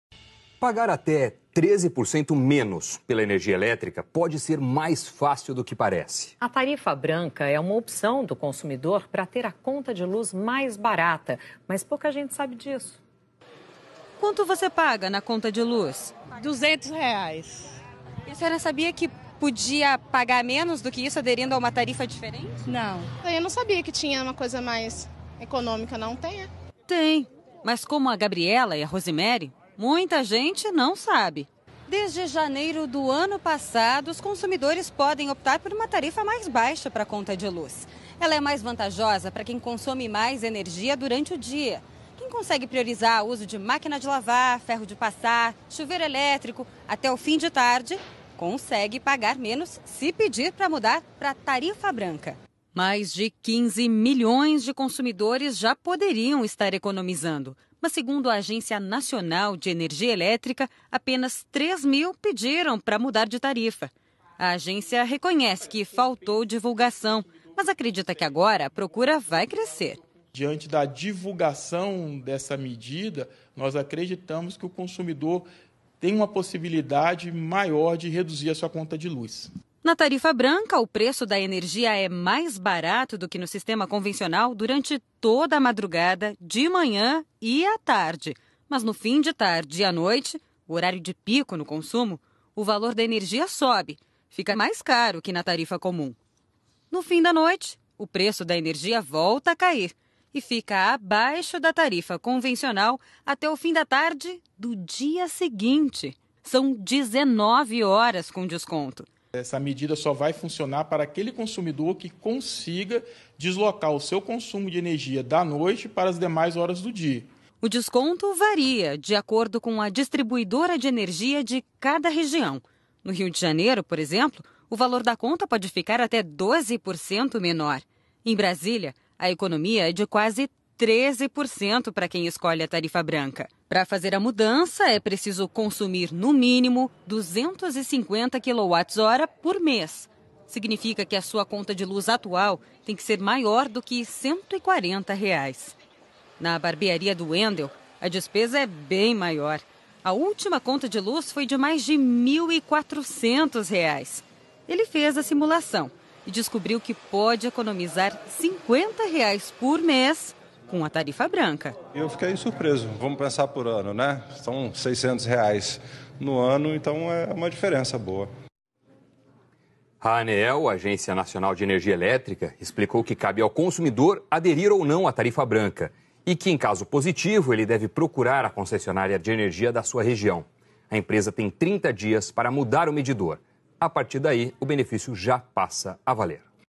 Como deixar a conta de luz mais barata até R$50,00 a menos.
Você sabia que pode pagar mais barato na conta de luz?
Veja como no vídeo.